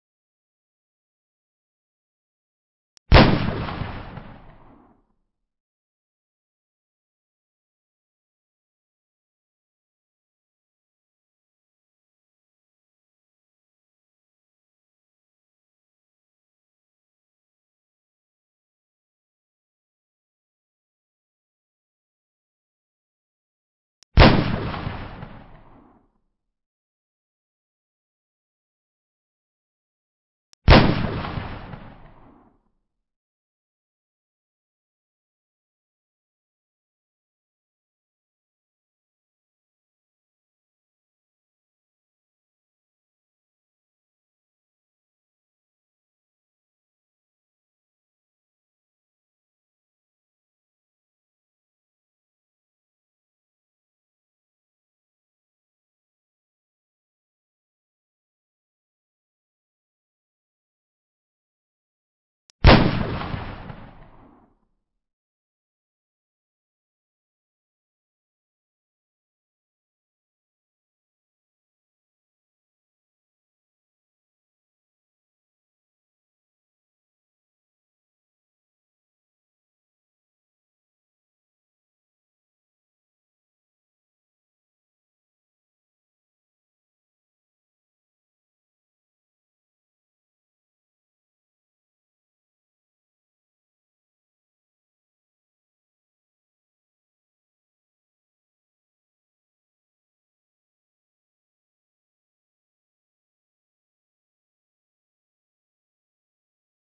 Two Prairie Hogs
Three hogs down and two recovered in middle of swamp, water about a foot deep all around.